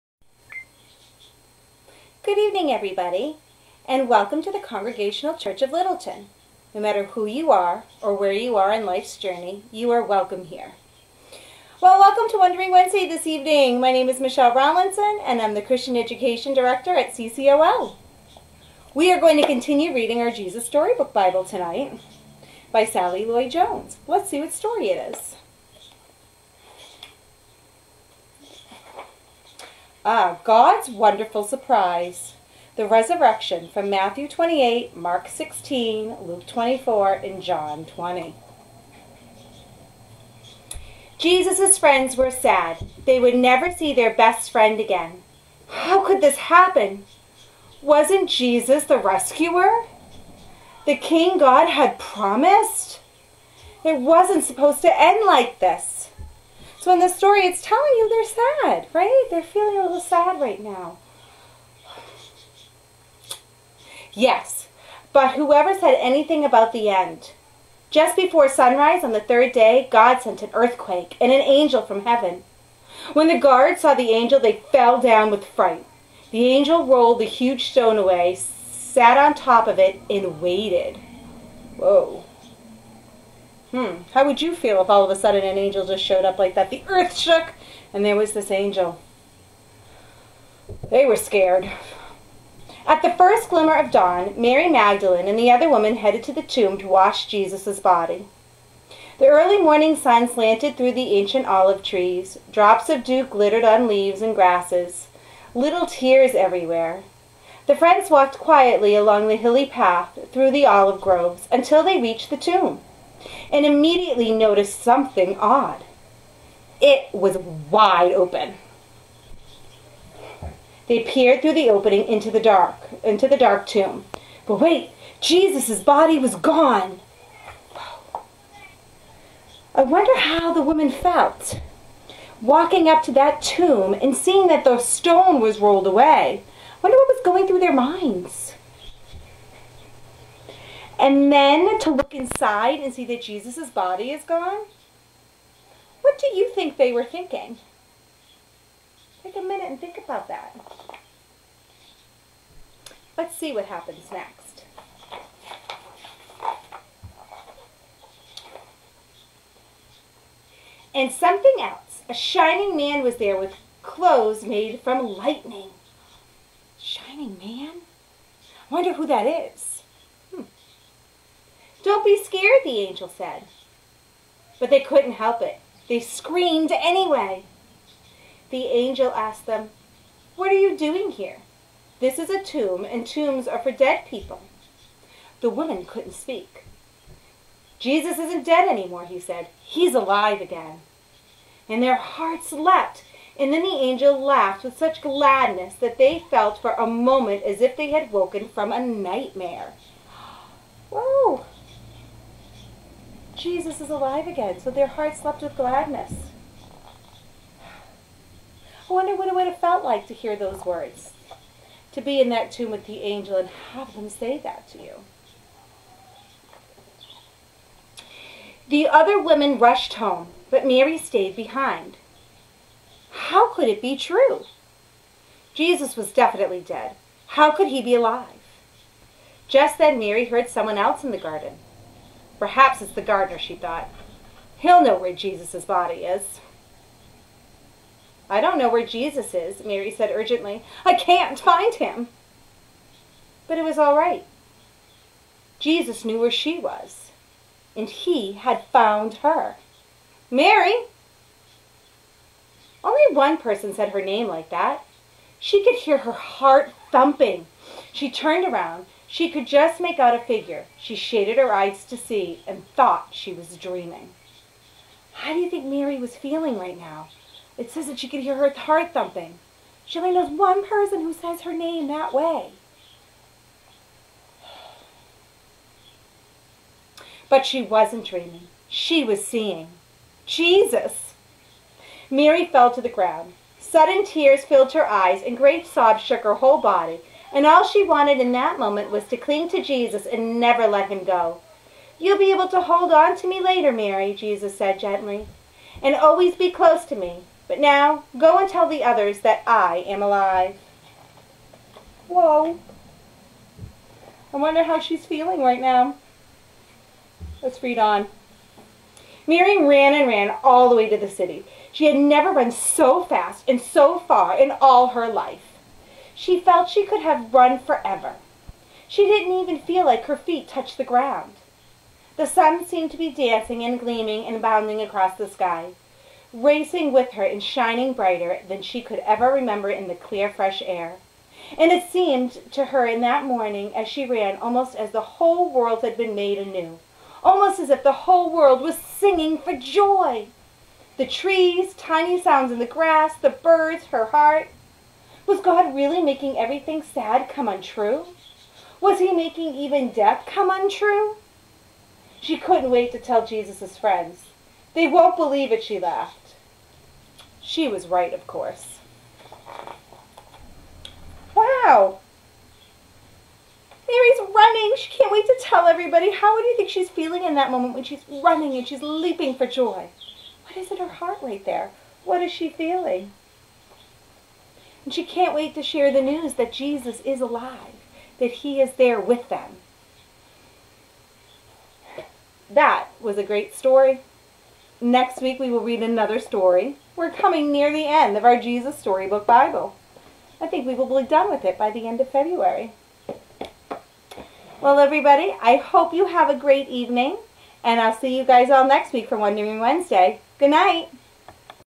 "God's Wonderful Surprise"- Wondering Wednesday
Jan 21; "God's Wonderful Surprise" The Resurrection from Matthew 28, Mark 16, Luke 24, John 20 reading from the Jesus Storybook Bible by Sally Lloyd-Jones